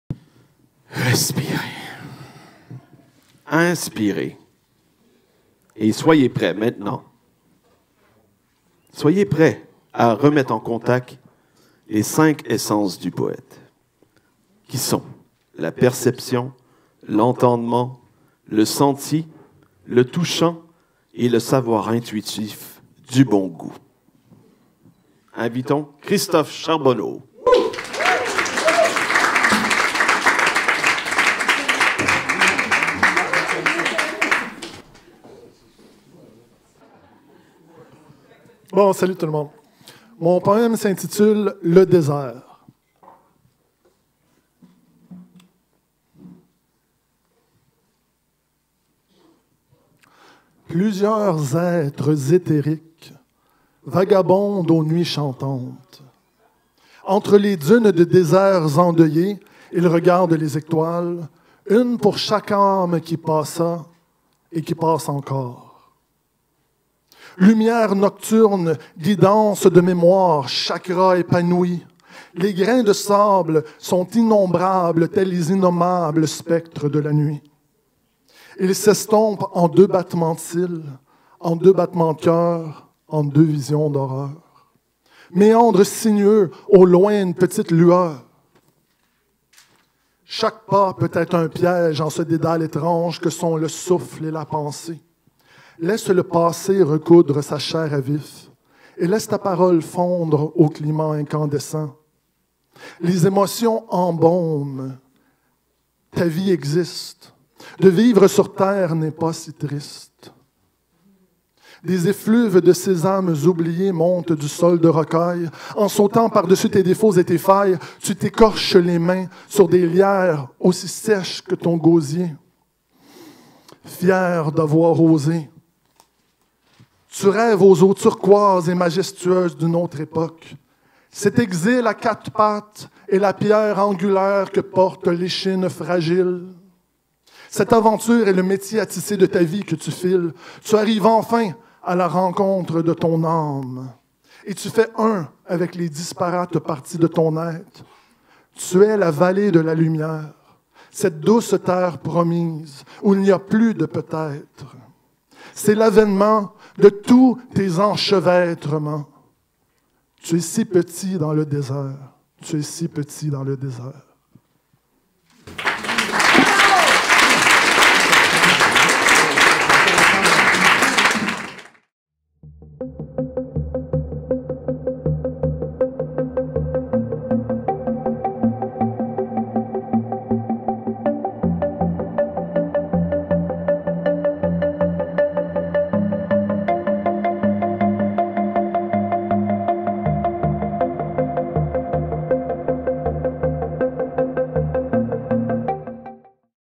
Performance complète de Christophe Charbonneau - Finale du Concours de poésie Antidote 2019
Performance complète de Christophe Charbonneau  à la finale du Concours de poésie Antidote 2019
Organisée et animée par Roger Stephen Blaise
Quai des brumes - Montréal - Québec - Le 2 mars 2019
© vidéo et photos : Akim Kermiche